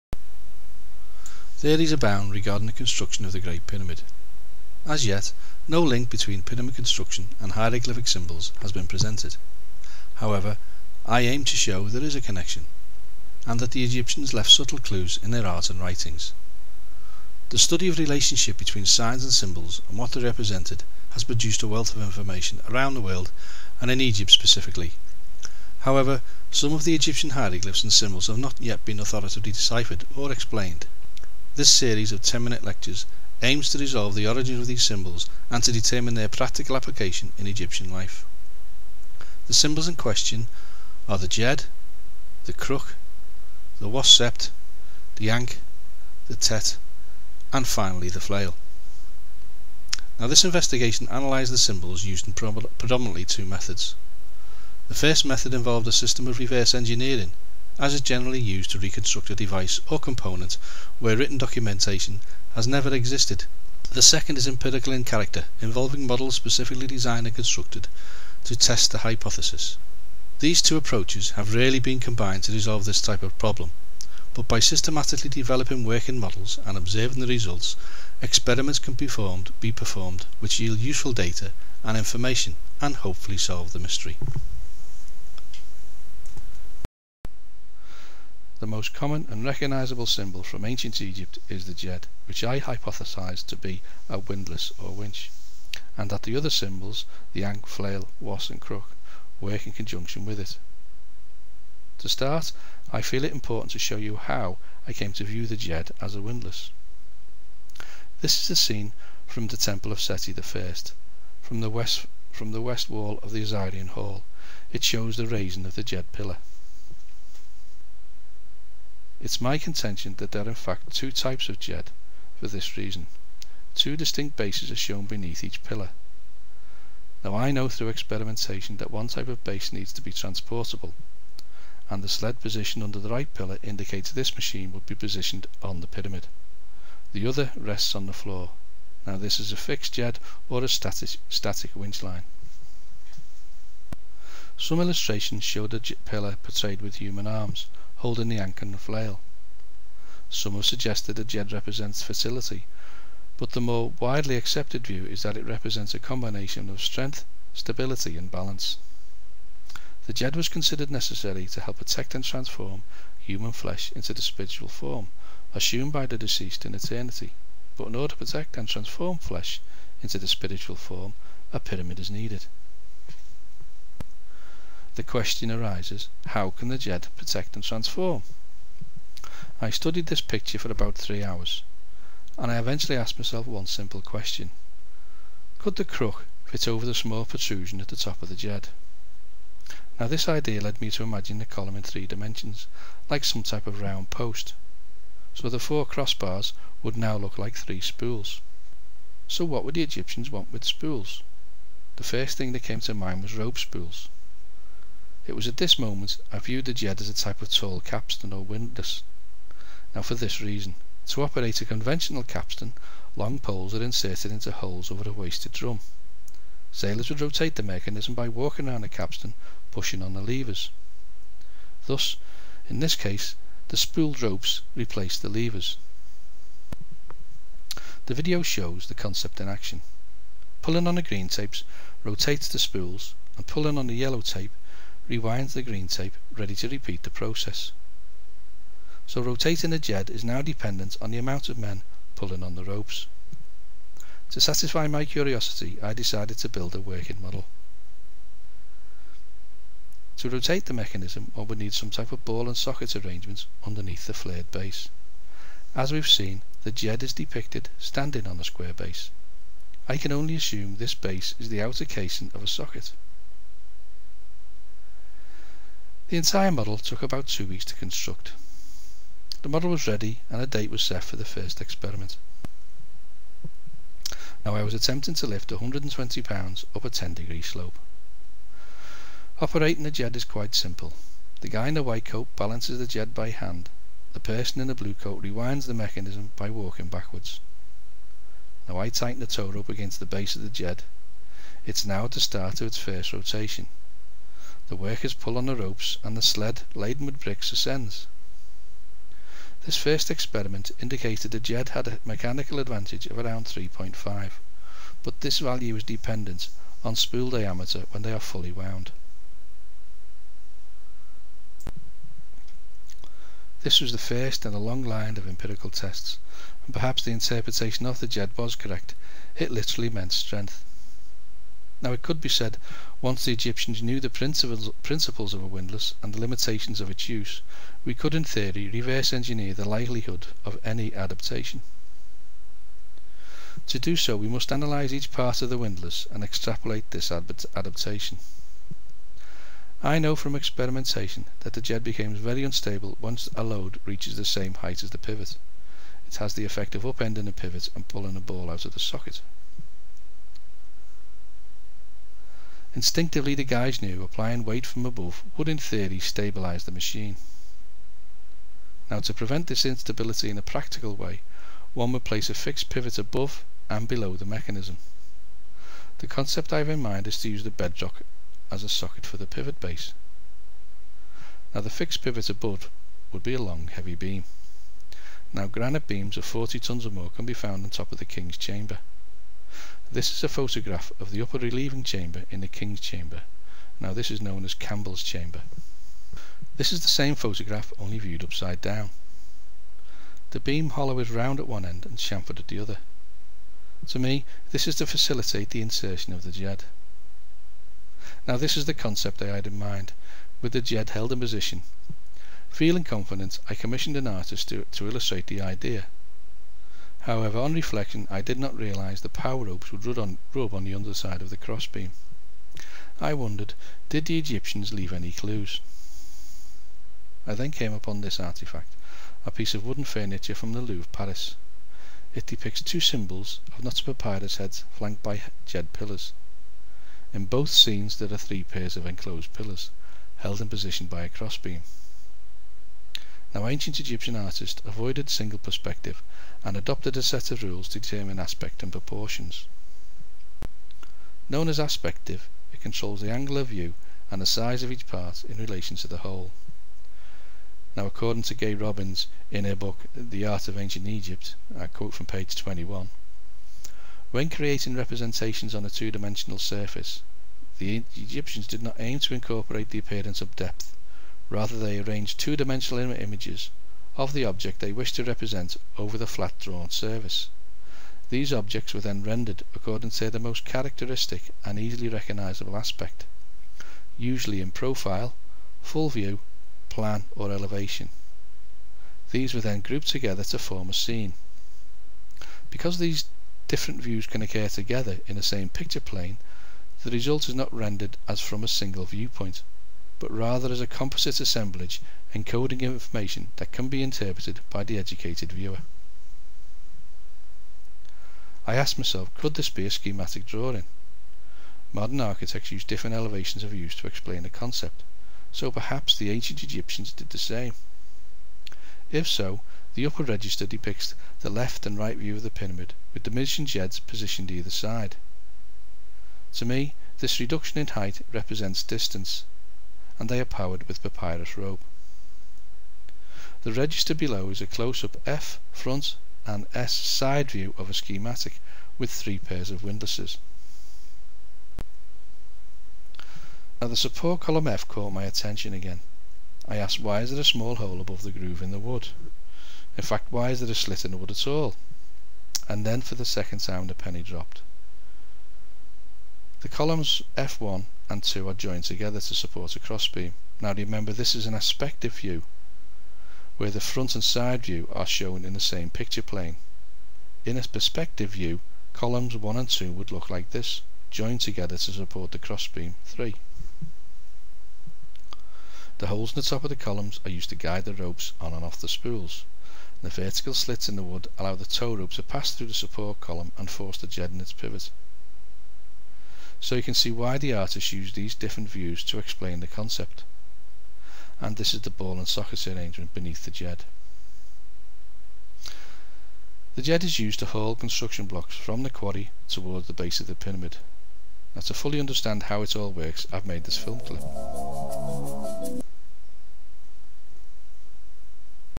part one pyramid construction theory 第一部ピラミッドの建設の理論
A new pyramid construction theory!!!Theories abound regarding the construction of the great pyramid; as yet no known link between pyramid construction and hieroglyphic symbols has been  presented. However, I aim to show there is a connection, that the Egyptians left subtle clues in there art and writings. ！新しいピラミッドの建設理論理論は偉大なピラミッドの建設に関する富む、ピラミッド建設や象形文字の記号の間にはまだありません既知のリンクとして表示されています。しかし、私は接続がエジプト人が芸術と著作の中で微妙な手がかりを残したこと、ある表示を目指しています。